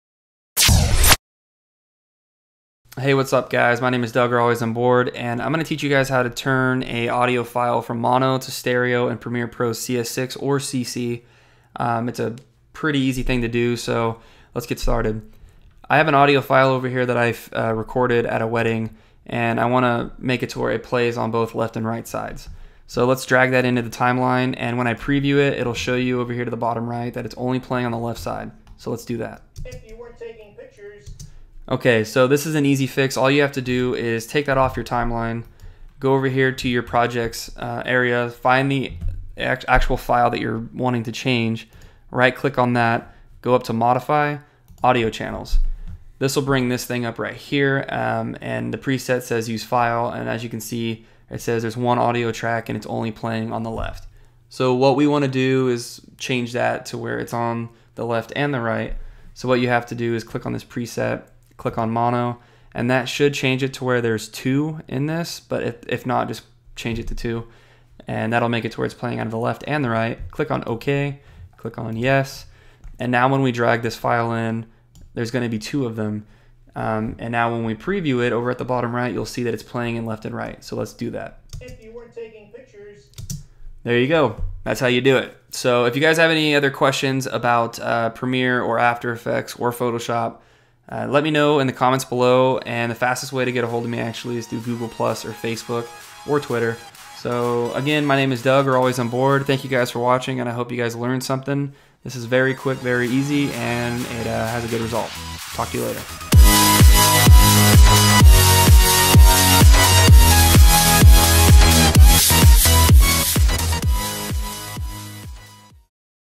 Mono to Stereo in Premiere Pro CC Tutorial
This tutorial shows you step by step on how to change mono to stereo in Adobe Premiere CC. It'd be awesome if you Rated this!